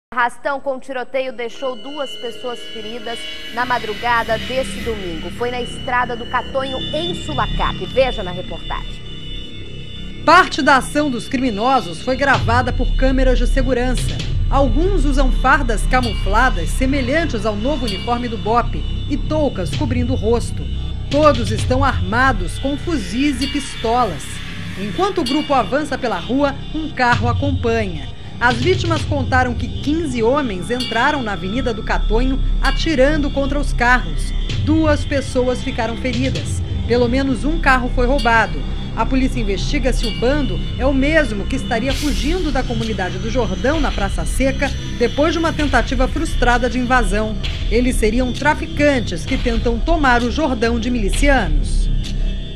Arrastão em Sulacap
UM ARRASTÃO COM TIROTEIO DEIXOU DUAS PESSOAS FERIDAS NA MADRUGADA DESSE DOMINGO./ FOI NA ESTRADA DO CATONHO, EM SULACAP.